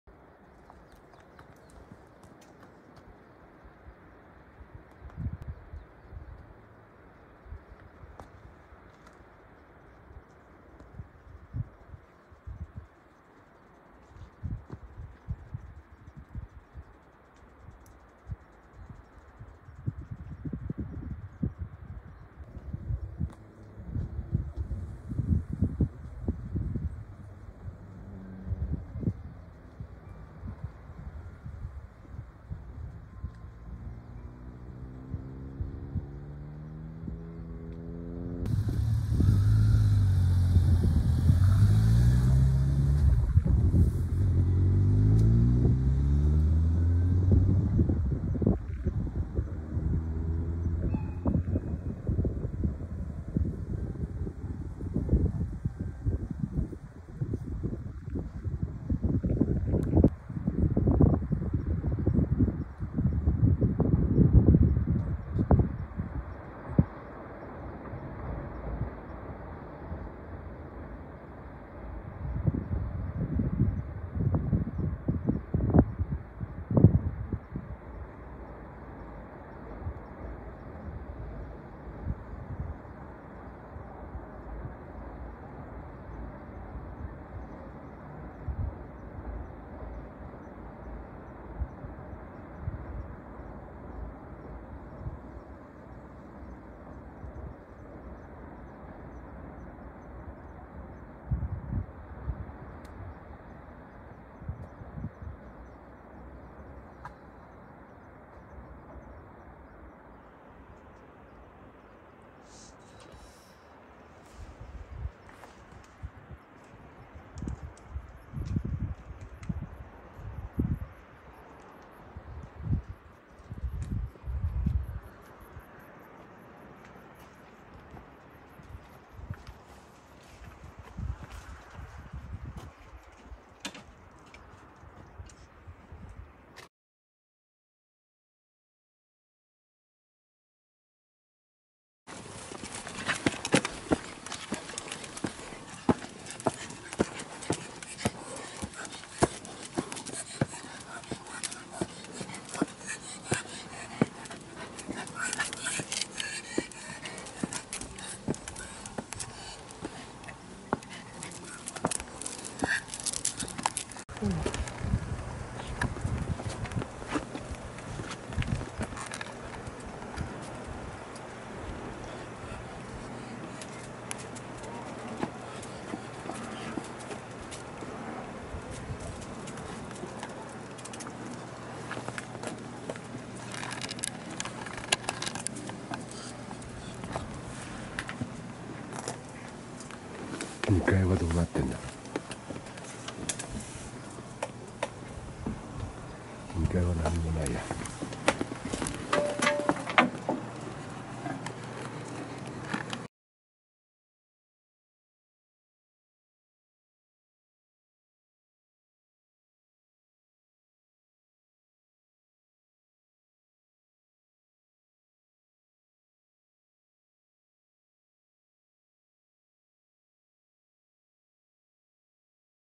保津峡展望台からの眺めと小倉山カフェからの保津峡
小倉山カフェはテラス席犬同伴可
嵐山高雄パークウェイ内
竹林の小径で散歩の後、ワン遊ランドの帰途小倉山カフェ利用